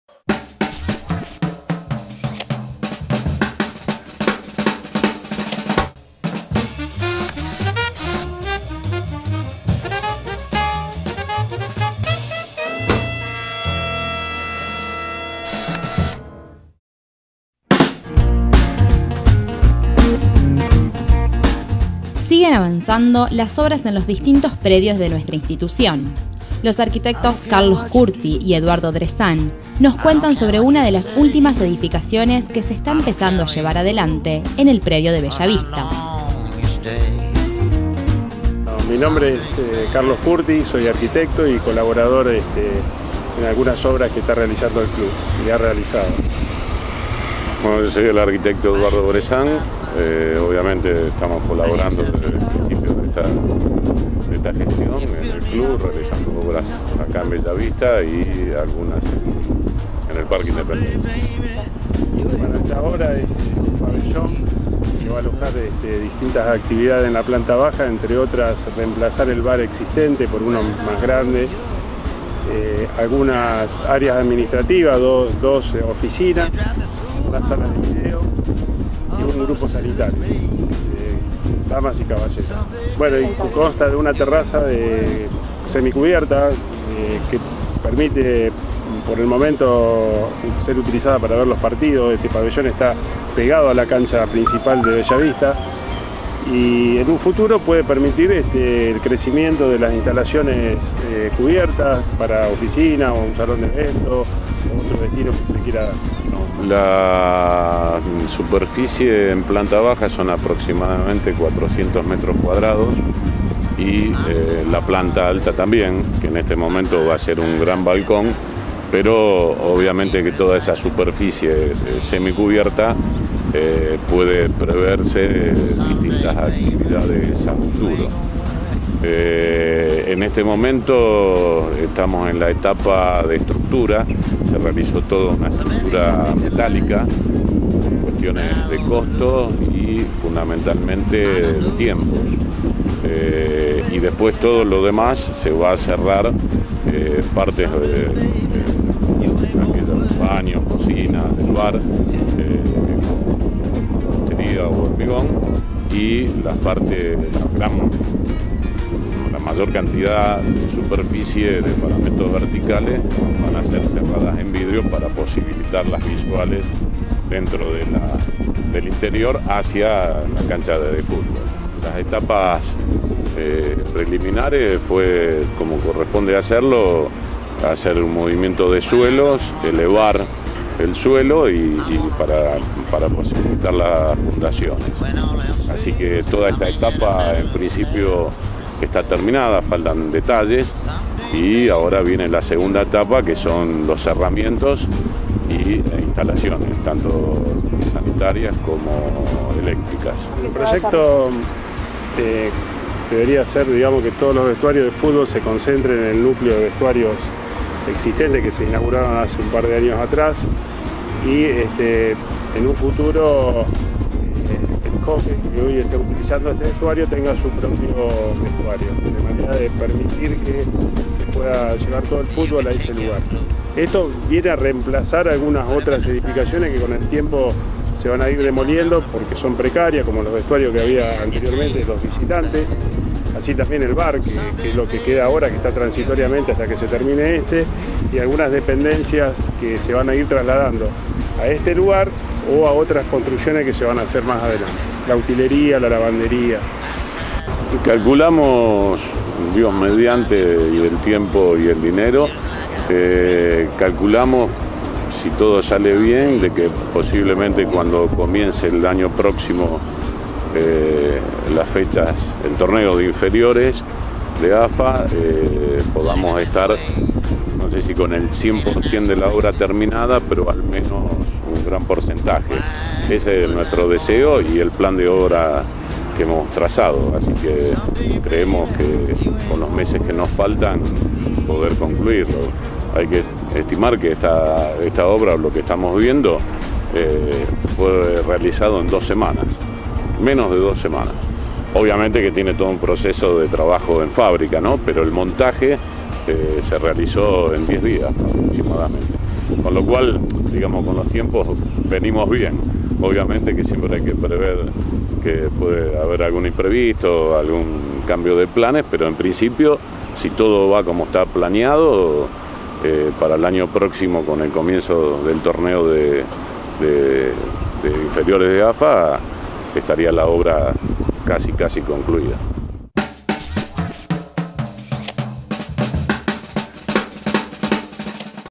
Nuevas obras en Bella Vista
Se está construyendo en el predio una importante edificación de dos plantas que brindará mayor comodidad tanto para quienes trabajan diariamente en el predio como para los hinchas que lo visiten. Entrevista con los arquitectos Carlos Bressan y Carlos Curti.